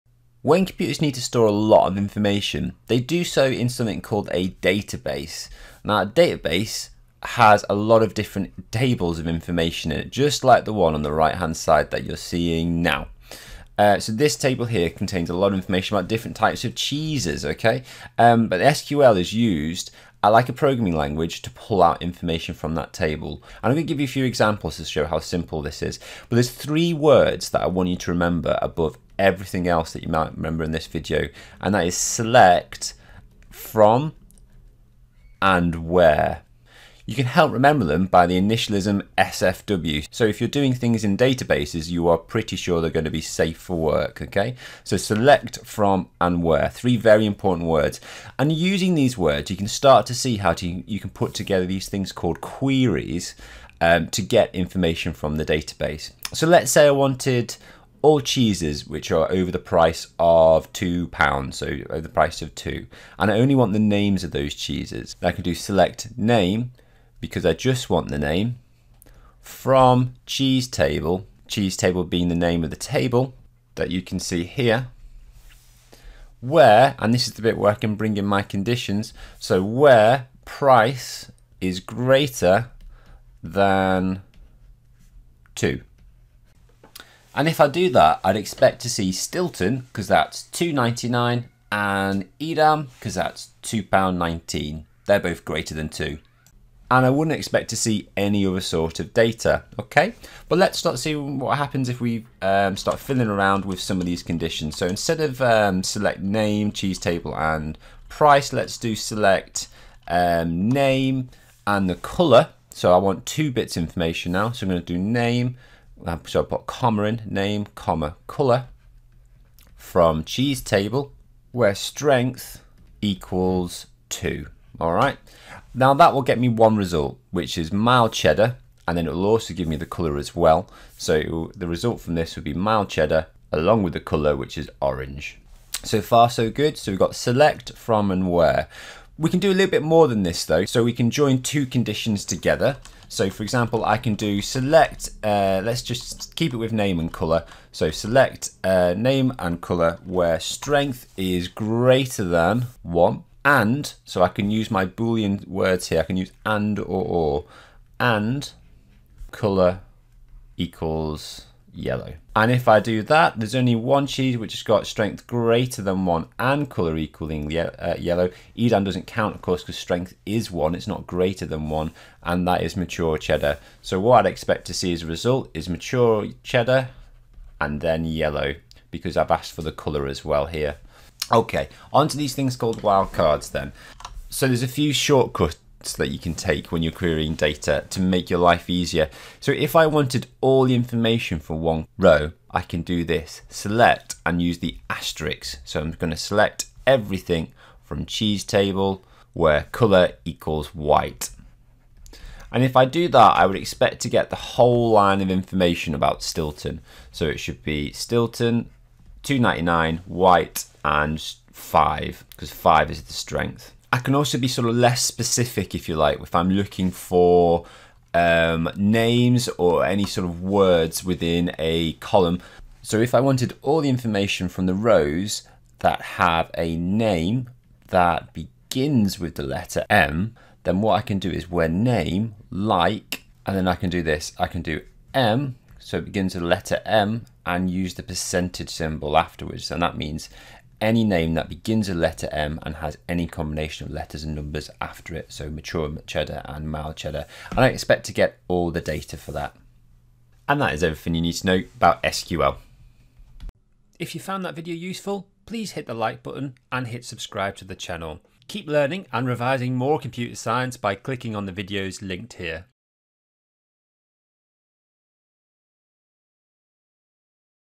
2.2.3 SQL - Revise GCSE Computer Science
Revise how to use an SQL command to get data from a database, find out about the three essential keywords: SELECT, FROM, and WHERE.  Revise this important skill for GCSE Computer Science!
0:00 SQL and Databases
0:25 SELECT...FROM...WHERE
0:56 Running an SQL Query
1:57 Using multiple conditions
3:09 Using wildcards (* and %)